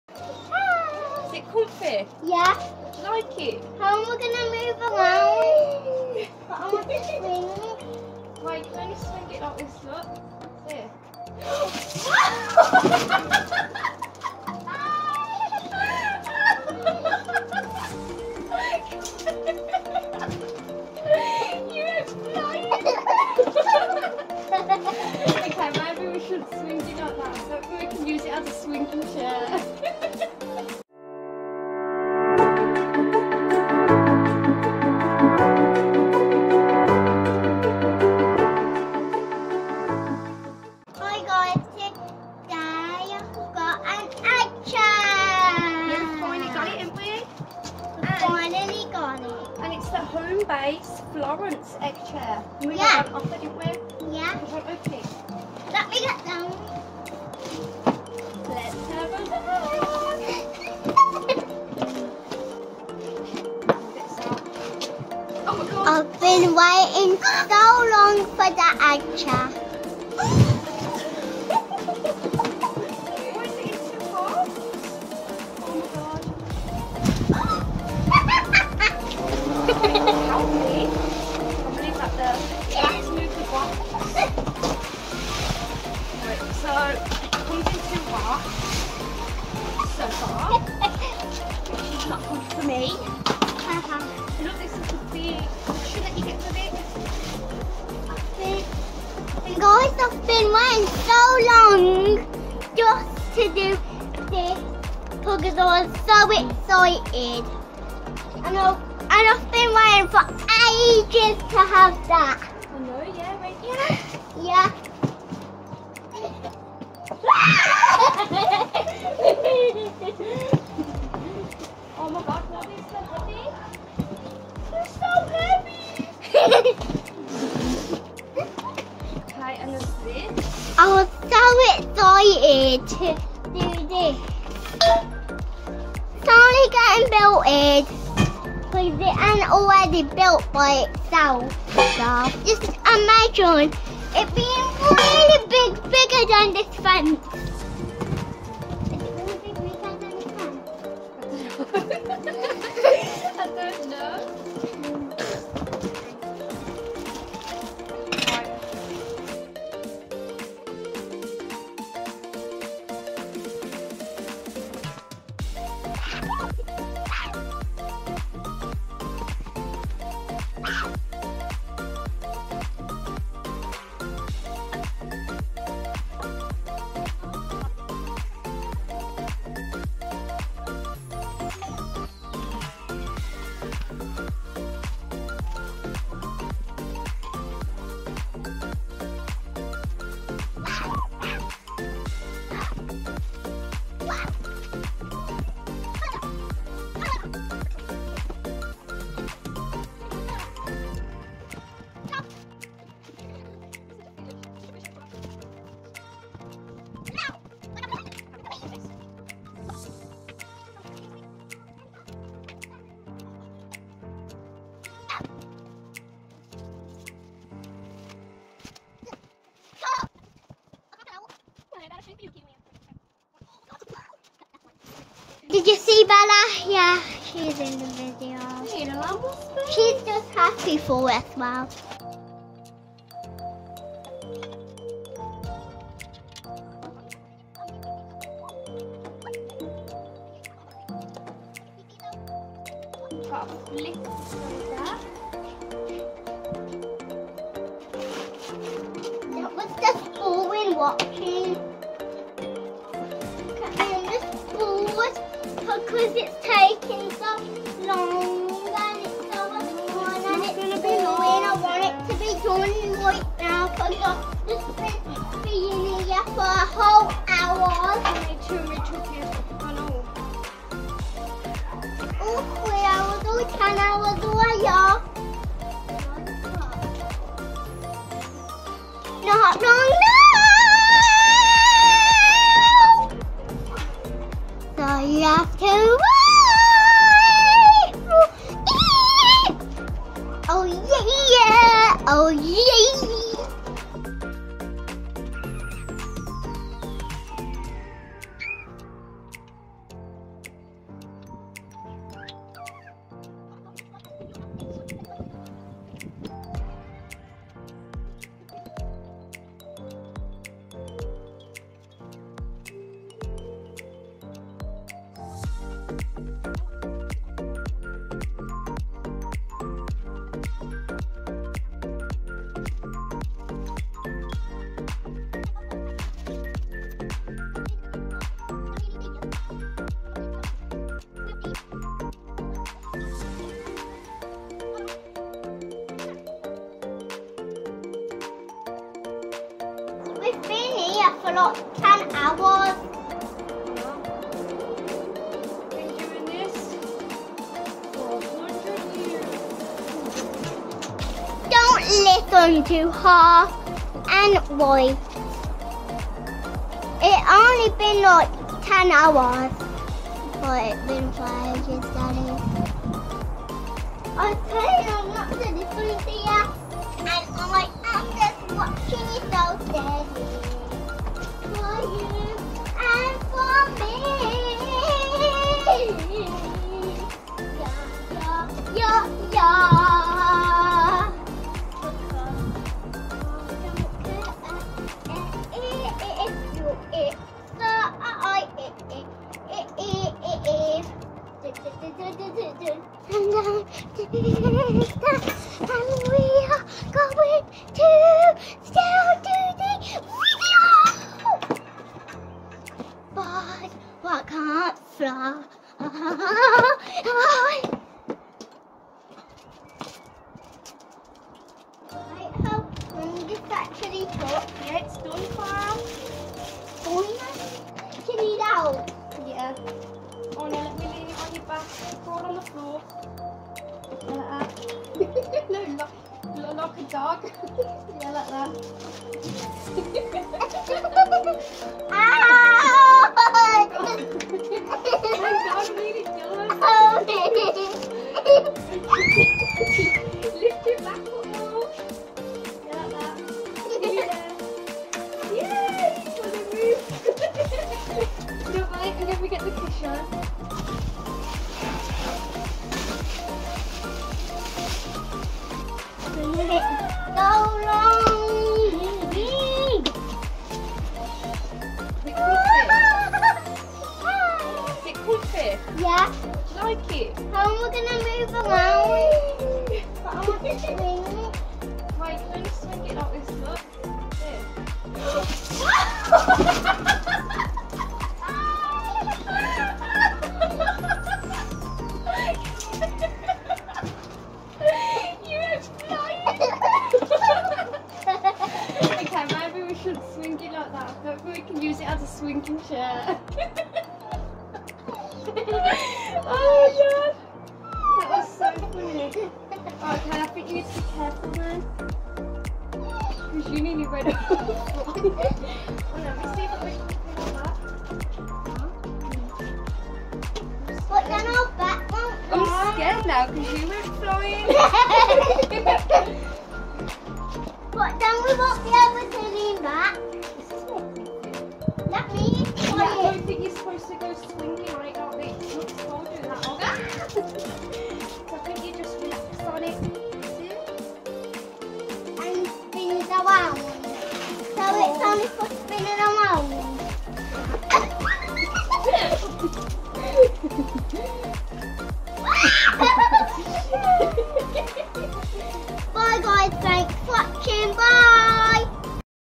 Homebase Florence Hanging Egg Chair, Unboxing & Assembly Review Kyratopia
@homebase Florence egg chair review & assembly, so happy to finally snap this up during sales, taken me 60 minutes to assemble, easy to follow instructions very happy :D